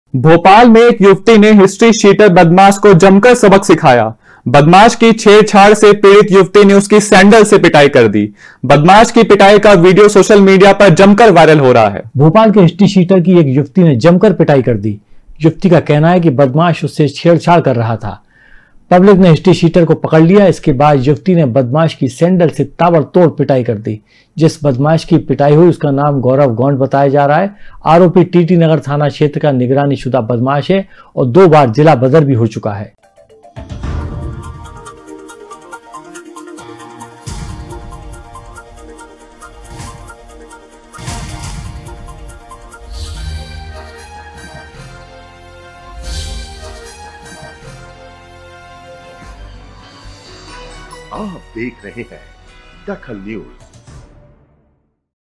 DAKHAL NEWS युवती ने बदमाश को सिखाया सबक
युवती ने बदमाश को सिखाया सबक
छेड़छाड़ करने पर सैंडल से पीटा